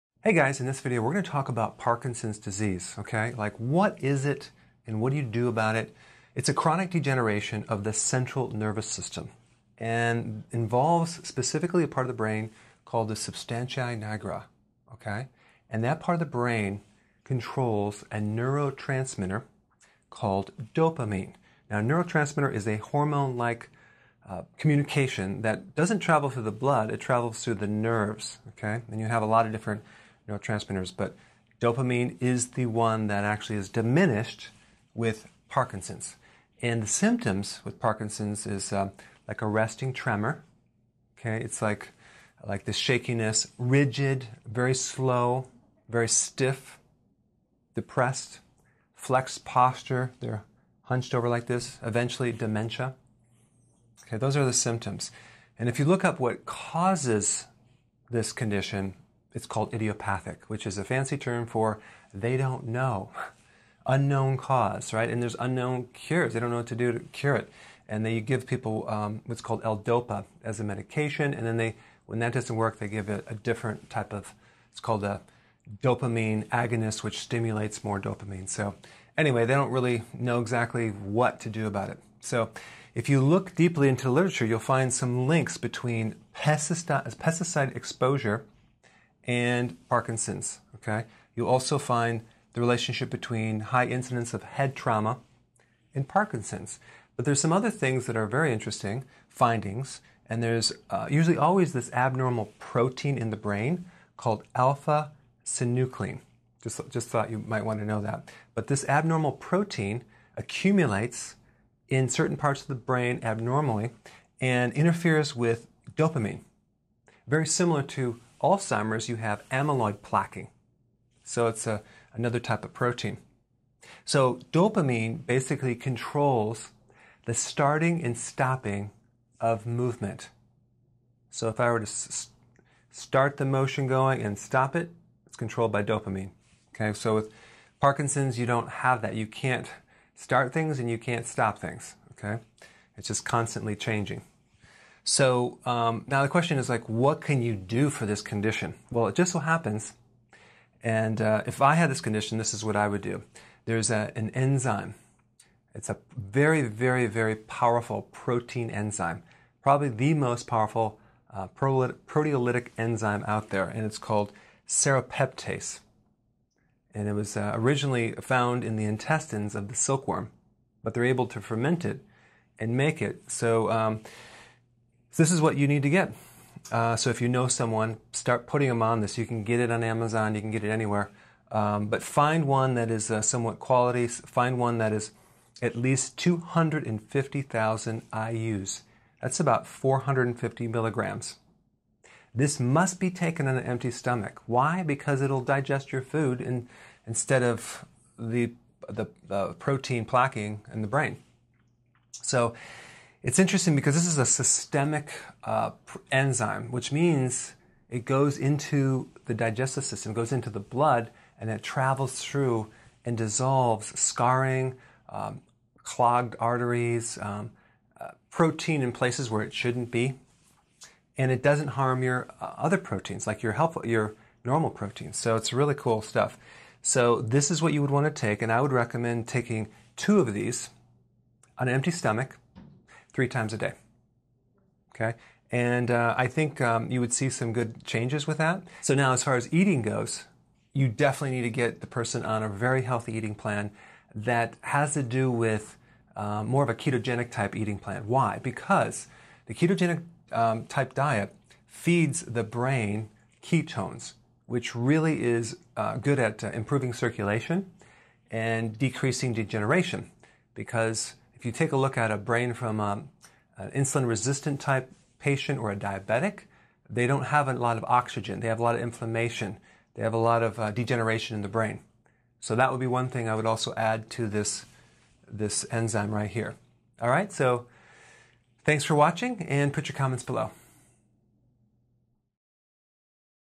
What is Parkinson's Disease and What Causes this Disease? - Explained by Dr. Berg
Learn more about Parkinson's disease: what it is, what causes it, and what you can do about it.

Timestamps
0:00 What is Parkinson's disease?
0:35 Parkinson's symptoms
0:55 What causes Parkinson's disease?
2:25 Natural remedies for Parkinson's disease symptoms

Today I want to cover Parkinson's disease. Parkinson's disease is a chronic degeneration of the central nervous system. It involves a specific part of the brain that controls dopamine. Dopamine is diminished with Parkinson's disease.

Symptoms of Parkinson's disease:
 • Resting tremor
 • Rigidness
 • Slowness
 • Stiffness
 • Flexed posture
 • Dementia

The cause of Parkinson's disease is unknown. There is also no cure for Parkinson's disease. Some pieces of literature show links between pesticide exposure and Parkinson's disease and a relationship between head trauma and Parkinson's disease.

Other interesting findings show that there is almost always a specific abnormal protein in the brain of Parkinson's patients. This abnormal protein can interfere with dopamine.

Dopamine controls the starting and stopping of movement. With Parkinson's disease, you can't start and stop movements.

Potential remedies to help with Parkinson's disease symptoms:
 • Serrapeptase (on an empty stomach)
 • Healthy Keto diet